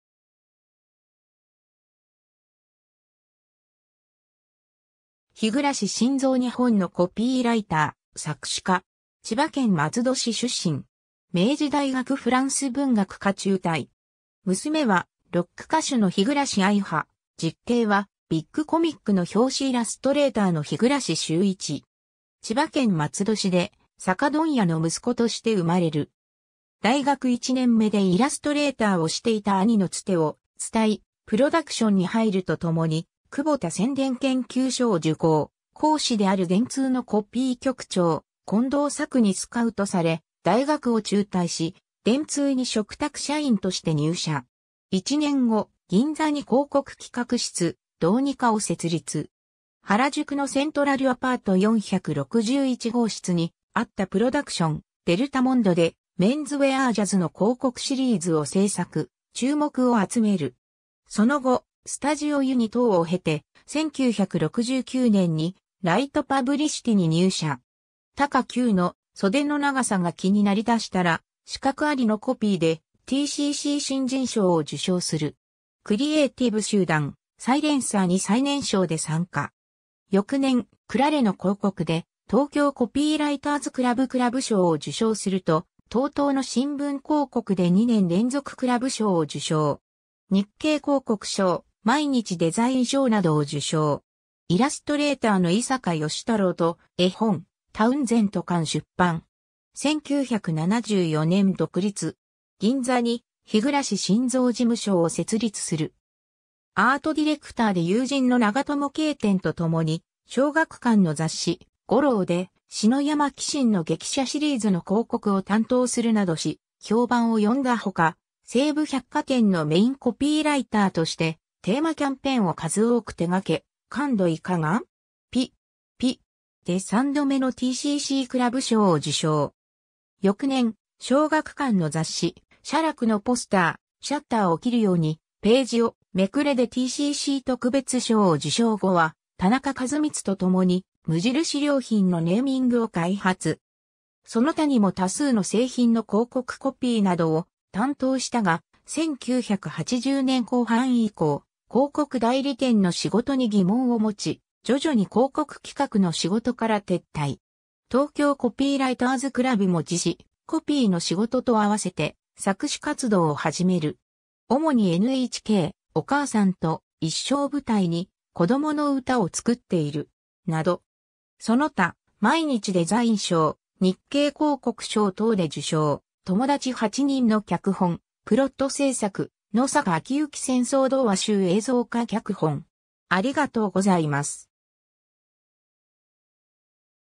日暮真三
日暮 真三（ひぐらし しんぞう、1944年3月1日 - ）日本のコピーライター、作詞家。
千葉県松戸市出身。
明治大学フランス文学科中退。
娘はロック歌手の日暮愛葉、実兄はビッグコミックの表紙イラストレーターの日暮修一。
 千葉県松戸市で酒問屋の息子として生まれる。
大学1年目でイラストレーターをしていた兄のツテを伝い、プロダクションに入ると共に、久保田宣伝研究所（現・宣伝会議コピーライター養成講座）を受講、講師である電通のコピー局長、近藤朔にスカウトされ、大学を中退し、電通に嘱託社員として入社。
1年後、銀座に広告企画室「ドーニカ」を設立。
原宿のセントラルアパート461号室にあったプロダクション、「デルタモンド」でメンズウェアーJAZZの広告シリーズを制作、注目を集める。
その後、「スタジオユニ」等を経て、1969年にライトパブリシティに入社。
taka-Qの「袖の長さが気になりだしたら、資格あり」のコピーでTCC新人賞を受賞する。
クリエイティブ集団「サイレンサー」に最年少で参加。
翌年、クラレの広告で東京コピーライターズクラブクラブ賞を受賞すると、TOTOの新聞広告で2年連続クラブ賞を受賞。
日経広告賞、毎日デザイン賞などを受賞。
イラストレーターの伊坂芳太良と絵本「タウンゼント館」出版。
1974年独立。
銀座に「日暮真三事務所」を設立する。
 アートディレクターで友人の長友啓典と共に、小学館の雑誌「GORO」で篠山紀信の激写シリーズの広告を担当するなどし、評判を呼んだ他、西武百貨店のメインコピーライターとしてテーマキャンペーンを数多く手がけ、「感度いかが？ピッ。ピッ。」で三度目のTCCクラブ賞を受賞。
翌年、小学館の雑誌「写楽」のポスター「シャッターを切るようにページをめくれ」でTCC特別賞を受賞後は、田中一光と共に無印良品のネーミングを開発。
その他にも多数の製品の広告コピーなどを担当したが、1980年後半以降、広告代理店の仕事に疑問を持ち、徐々に広告企画の仕事から撤退。
東京コピーライターズクラブも辞し、コピーの仕事とあわせて作詞活動を始める。
主にNHK「おかあさんといっしょ」を舞台に子供の歌をつくっている。
 など。
 その他、毎日デザイン賞、日経広告賞等で受賞 　「ともだち8にん」の脚本、プロット制作 　「野坂昭如戦争童話集」映像化脚本